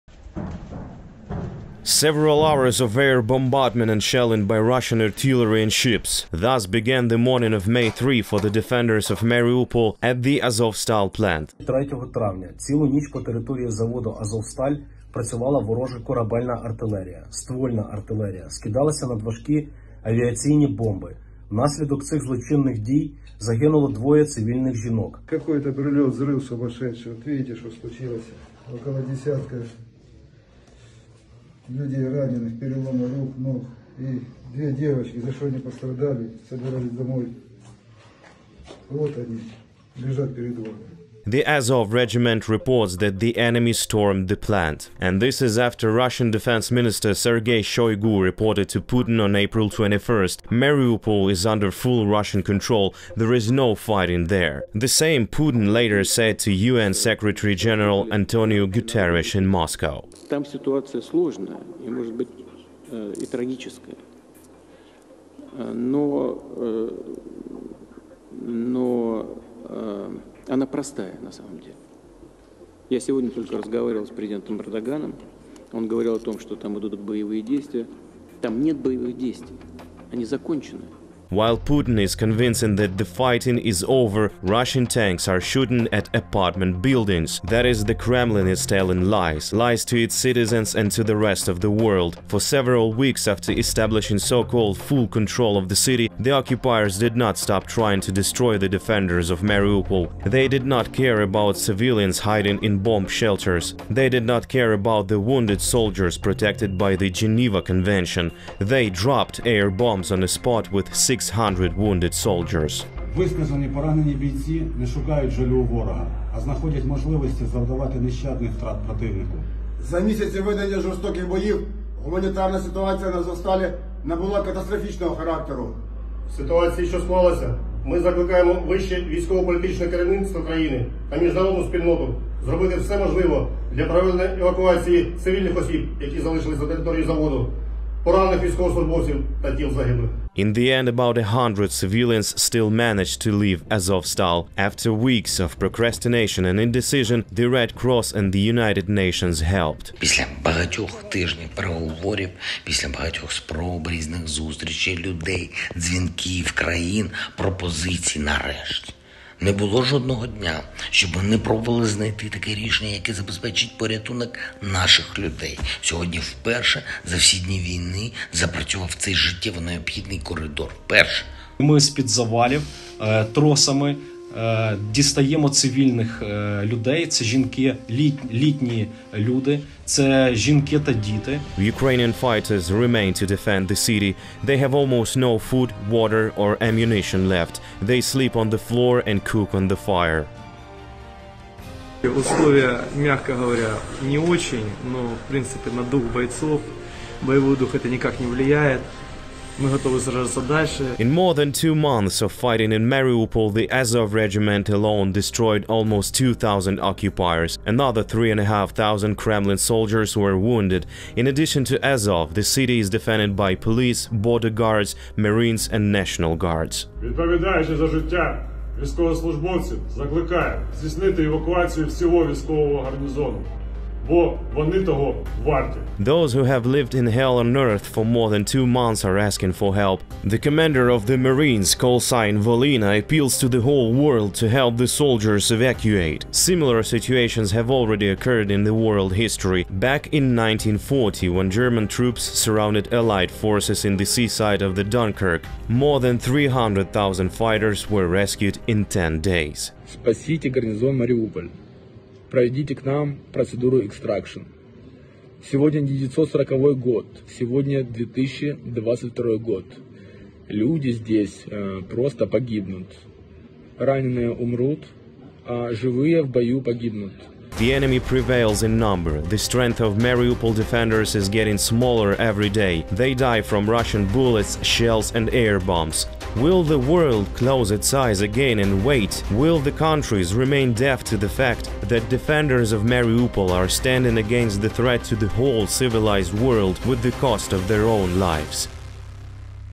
Mariupol today: hell made by russians
Two women dead, 10 civilians wounded during third day of the "russian cease fire" in Mariupol, Ukraine
Russians lied to UN Secretary General last week, russians lied to France president today.
There are still 100.000 people in the city.
There are still hundreds of civilians waiting for evacuation in Azovstal.
There is still genocide everyday